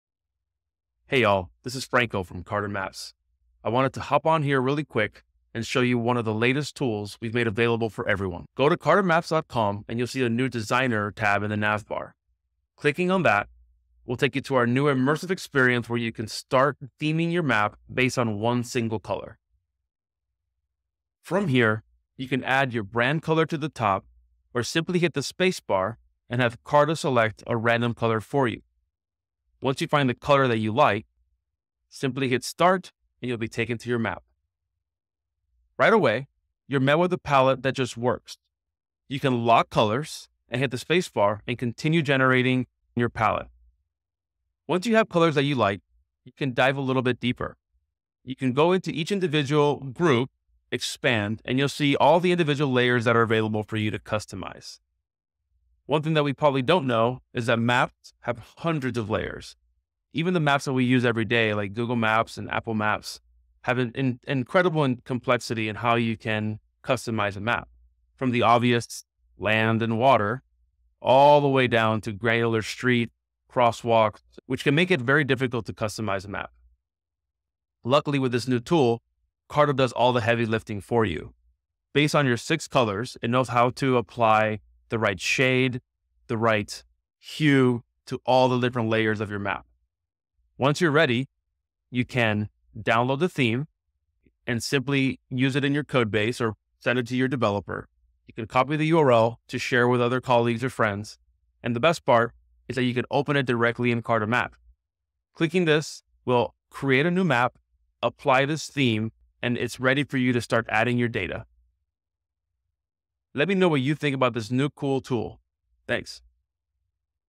Designer by Carta Map - The easiest way to create a theme for your map
Discover Carta Maps' latest tool, the Designer. Join me as I demonstrates how to create immersive map themes based on a single color. With a few clicks, you can add your brand color or let Carta select a random one for you. The intuitive palette simplifies the customization process, automatically applying the perfect shades and hues to various map layers, including streets, crosswalks, and more. Say goodbye to map customization challenges – Carta does all the heavy lifting! Once your ideal theme is ready, easily download it for your codebase or share the URL with colleagues and friends. Want to see it in action? Open it directly in Carta Maps and start adding your data hassle-free. Don't miss out on this cool new tool; try it now and let me know what you think!